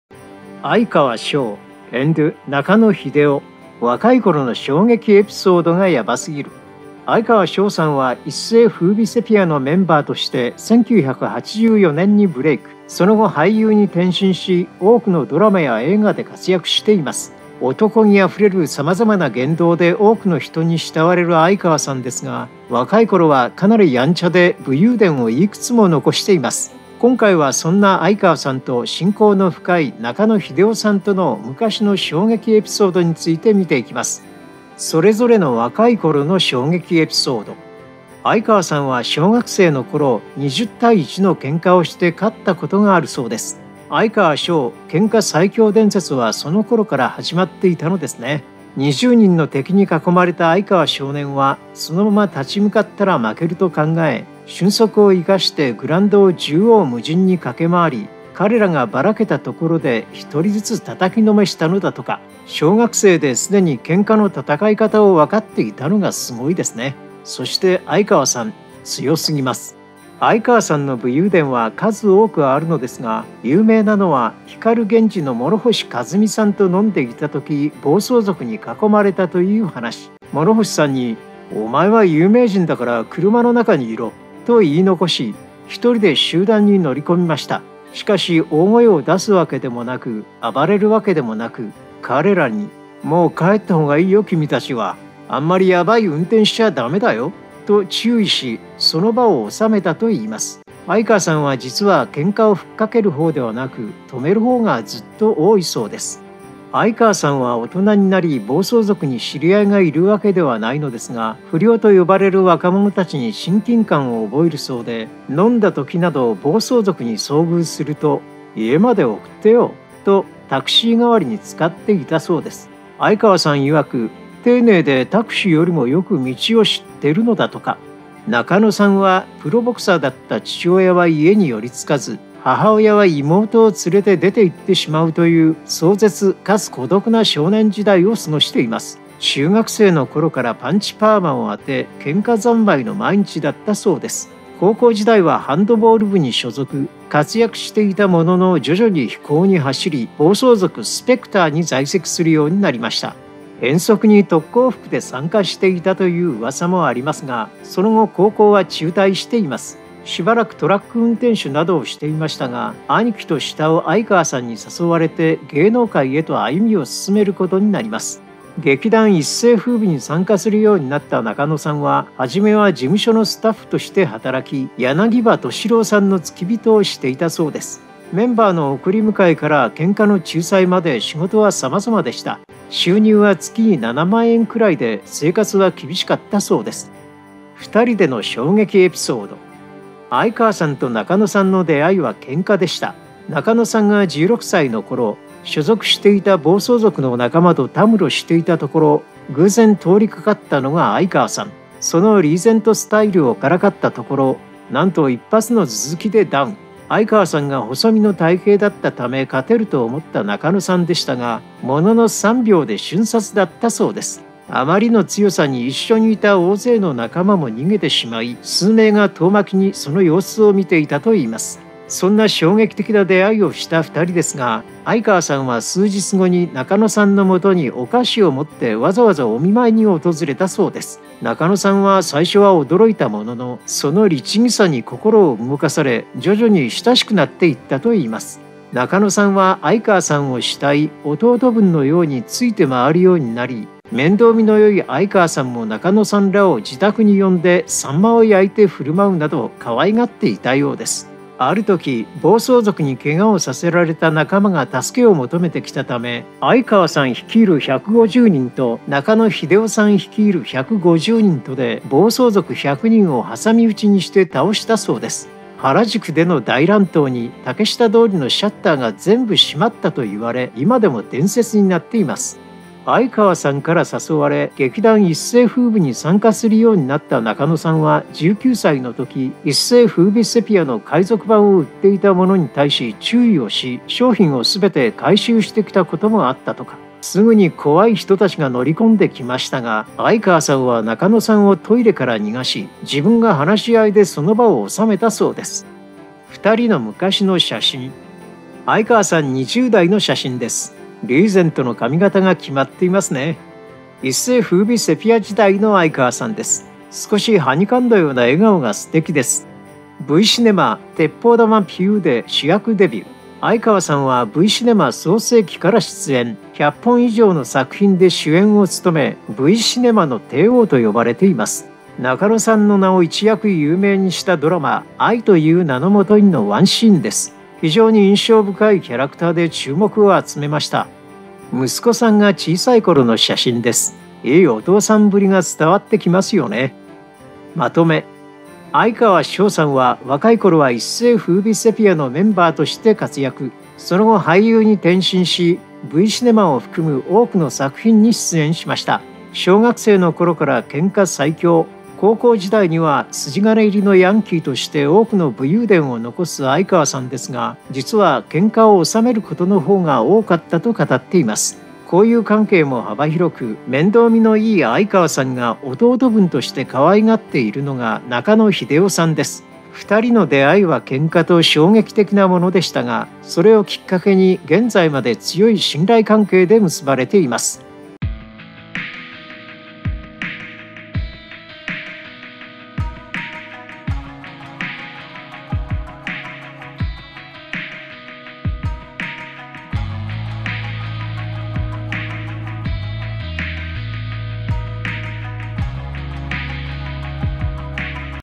哀川翔＆中野英雄、若い頃(昔)の衝撃エピソードがヤバすぎる…「暴走族の仲間とタムロしていたら…」
哀川翔さんは、一世風靡セピアのメンバーとして1984年にブレイク、その後俳優に転身し多くのドラマや映画で活躍しています。

男気あふれるさまざまな言動で多くの人に慕われる哀川さんですが、若い頃はかなりヤンチャで武勇伝をいくつも残しています。

今回はそんな哀川さんと、親交の深い中野英雄さんとの昔の衝撃エピソードについて見ていきます。